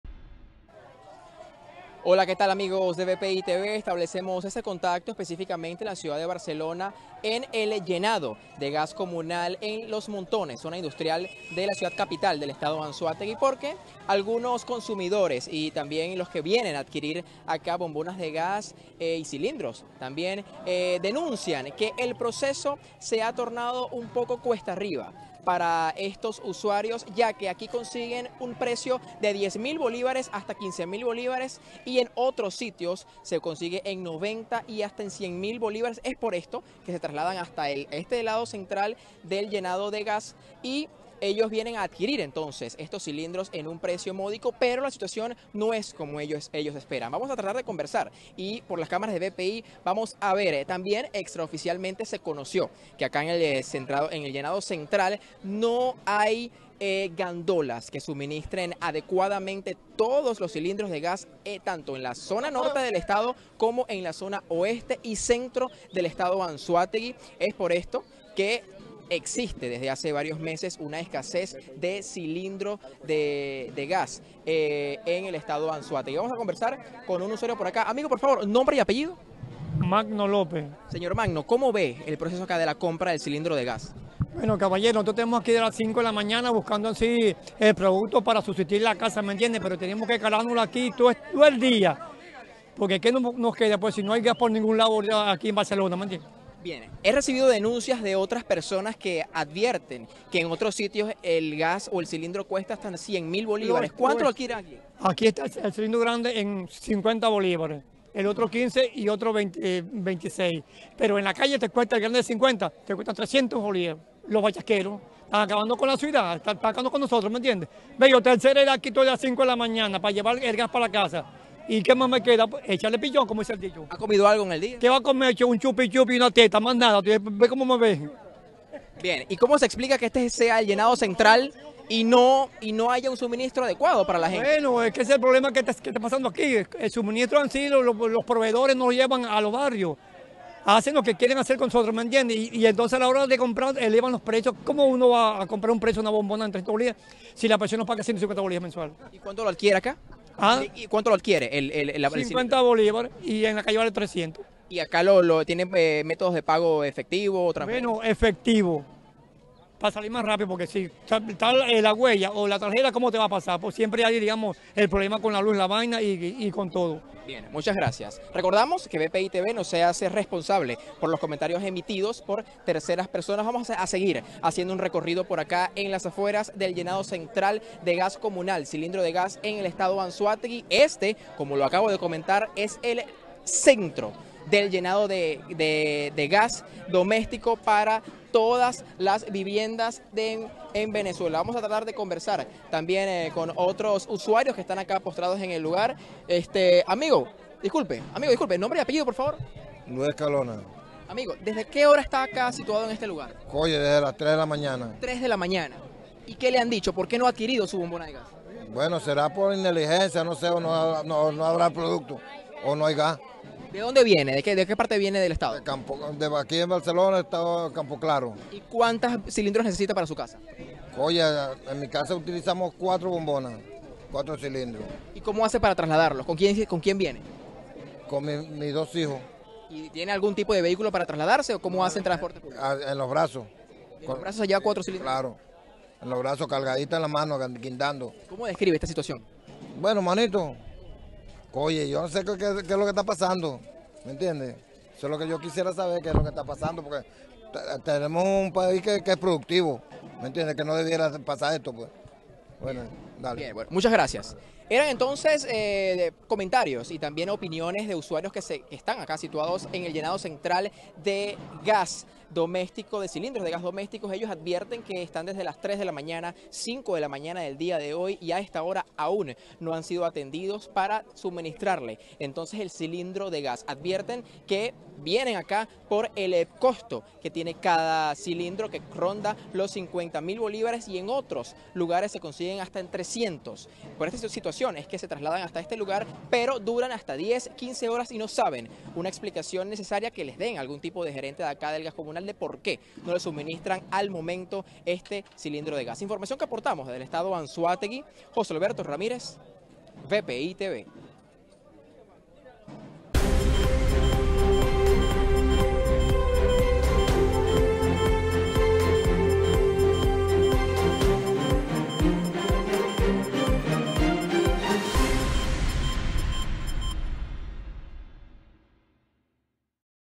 Anzoátegui - Adquirir el gas doméstico es una incertidumbre - VPItv
Magno López, habitante del estado Anzoátegui, indicó que para adquirir la bombona de gas en la localidad oriental es una problemática.